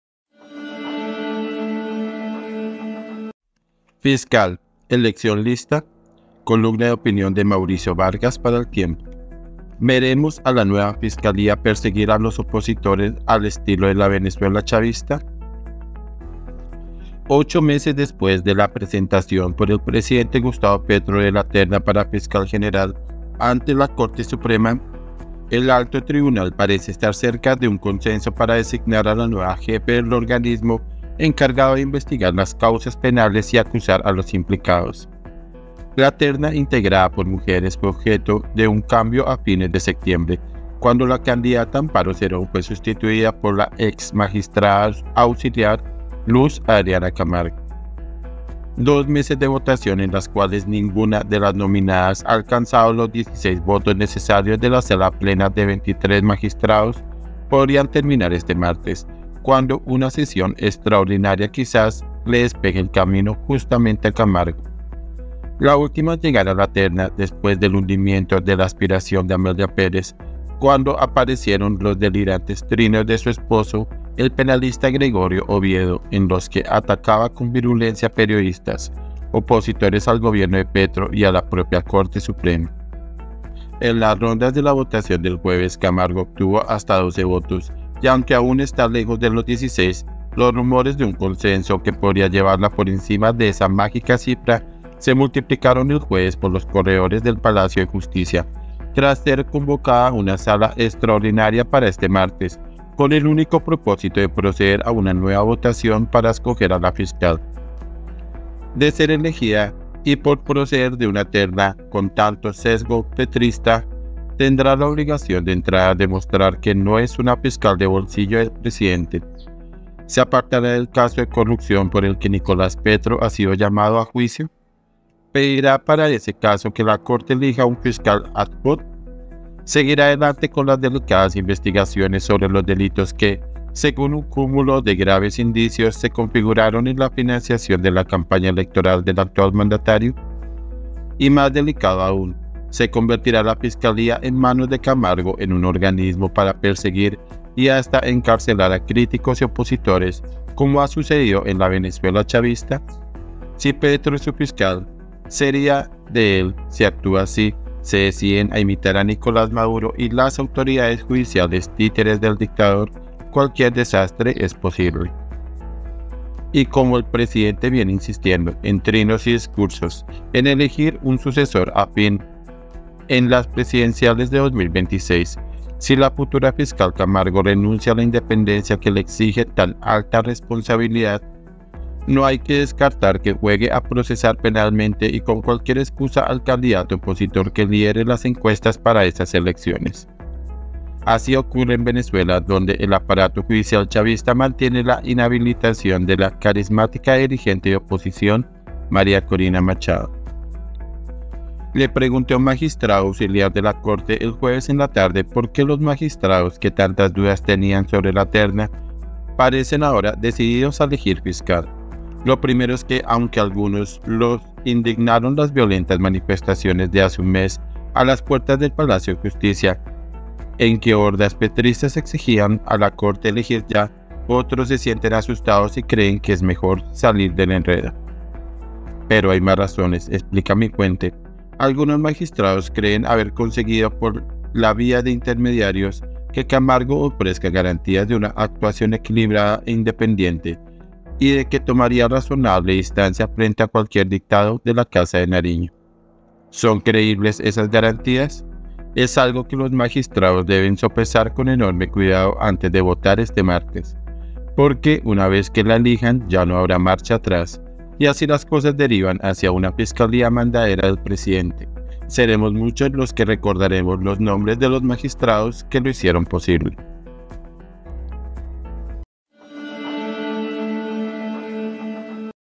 FISCAL: ¿ELECCIÓN LISTA? ⛔ Opinión de Mauricio Vargas
Ocho meses después de la presentación por el presidente Gustavo Petro de la terna para fiscal general, ante la Corte Suprema, el alto tribunal parece estar cerca de un consenso para designar a la nueva jefa del organismo encargado de investigar las causas penales y acusar a los implicados. La terna, integrada por mujeres, fue objeto de un cambio a fines de septiembre, cuando la candidata Amparo Cerón fue sustituida por la exmagistrada auxiliar Luz Adriana Camargo.
columna MAURICIO VARGAS